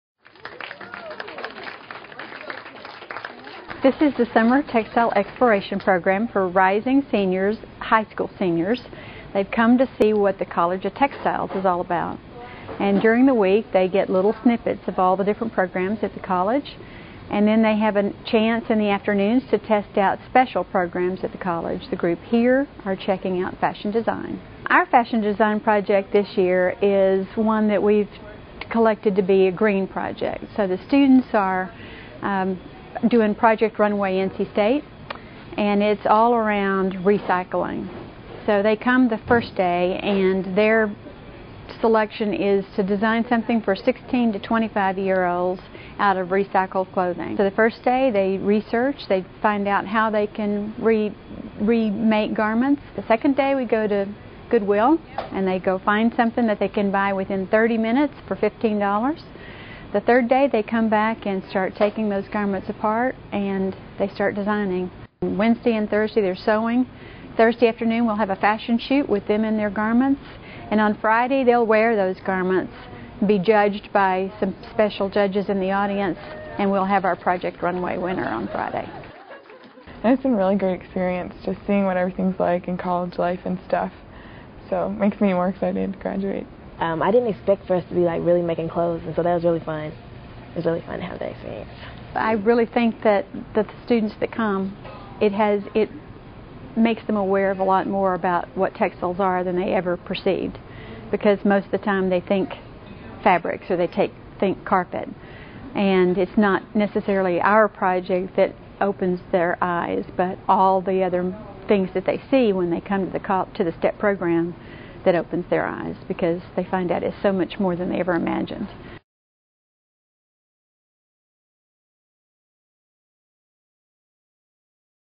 Project Runway
It's like Project Runway, Wolfpack-style.

High school seniors with an interest in fashion design came to the NC State College of Textiles earlier this summer to follow their dreams -- researching, constructing and modeling original clothing designs under the watchful eye of Summer Textile Exploration Program {STEP] professors and leaders.

"I really think that for the students that come, it makes them aware of a lot more about what textiles are than they ever perceived," said Dr. Cindy Istook, who specializes in apparel production and development at NC State. "It's not necessarily our project that opens their eyes, but all of the other things that they see when they come to the STEP program that opens their eyes, because they find that it's so much more than they ever imagined."

The College of Textiles consists of four one-week sessions for rising high school seniors. STEP was started in the early 1970's and was previously known as the Textiles Summer Research Program, before its name was changed in 2000. Each week, up to 28 students are chosen based on their academic success, extracurricular activities and their interest in NC State and science-related fields. Participants dedicate their time to a project pertaining to polymer and color chemistry, engineering, apparel, fabric design, computer science or business management, with each student choosing the project of his or her greatest interest.

"I didn't expect for us to be really making clothes," student participant Krystal Benson said. "It was really fun to have that experience."

Students also attend presentations on scholarships, the admissions process and take a campus tour. Over the past six years, approximately 200 STEP participants have enrolled at NC State, and all students completing STEP are automatically nominated for the North Carolina Textile Foundation's Centennial Scholarship process.